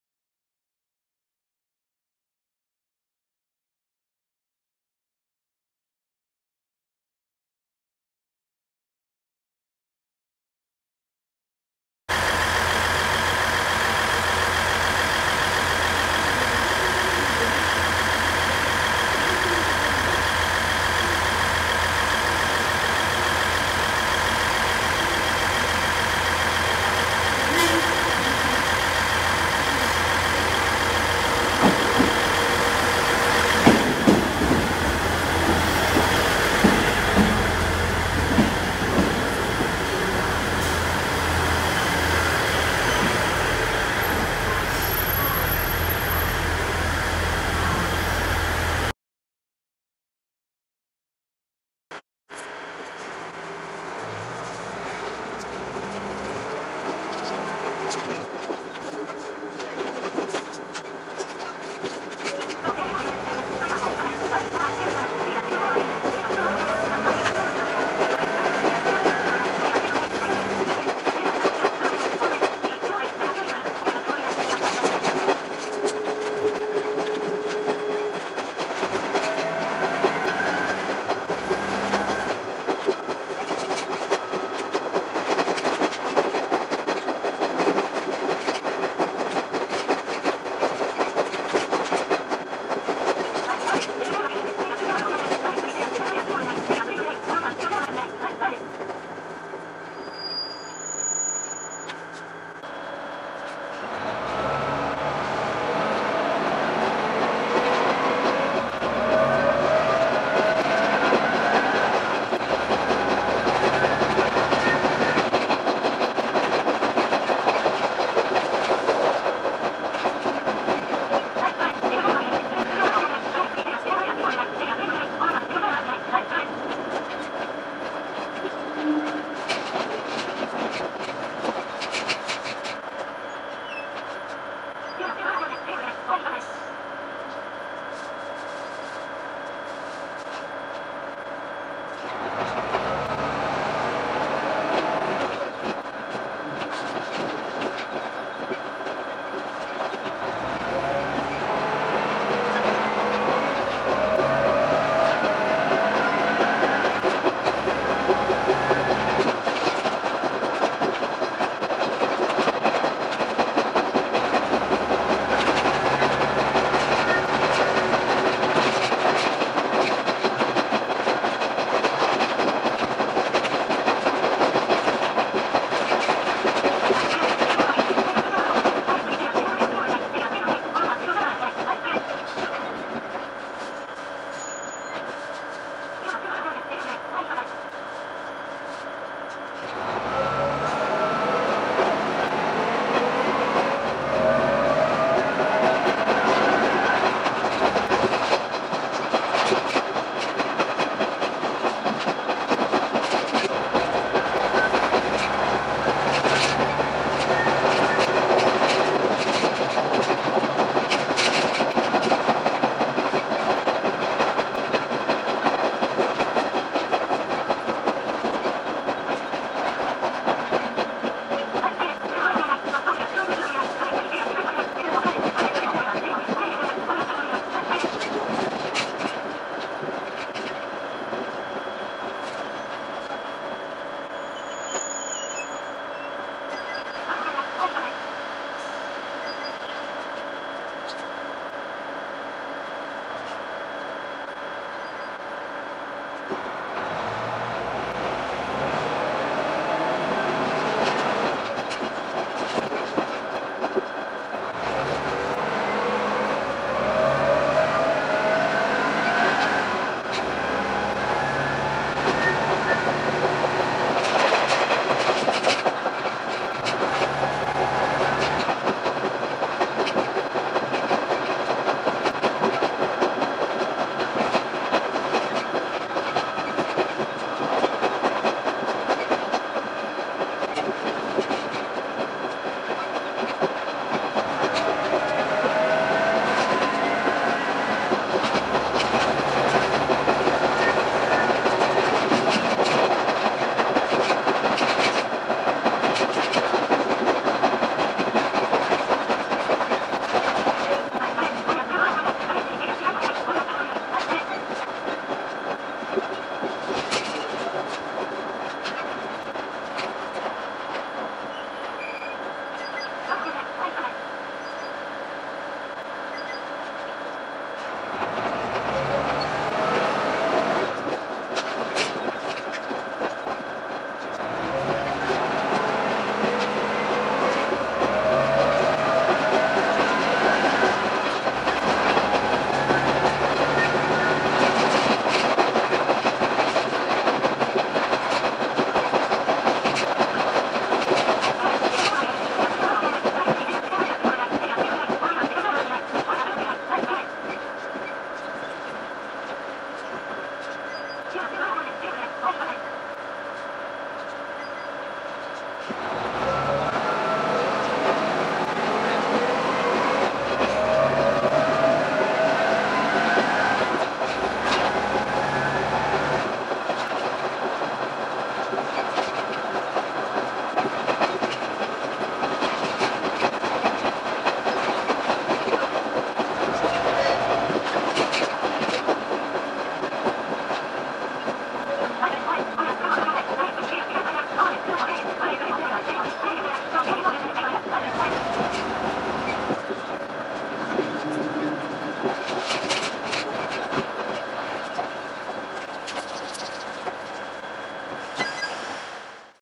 【6倍速後面展望】キハ110系 石巻線 石巻〜小牛田
石巻10:36発石巻線小牛田行きの車窓です。
乗車車両は、仙ココのキハ110系のキハ110-240です。